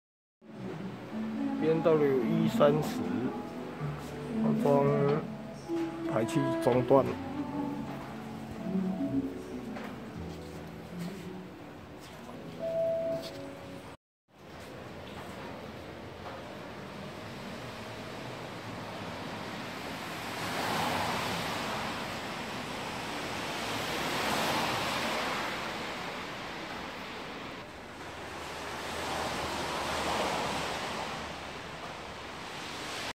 寶馬 BMW E30 318 排氣管中段改裝白鐵製品 51MM燒焊製作，消音砲彈安裝施工
台中 改裝排氣管，R2車業，沙鹿阿吐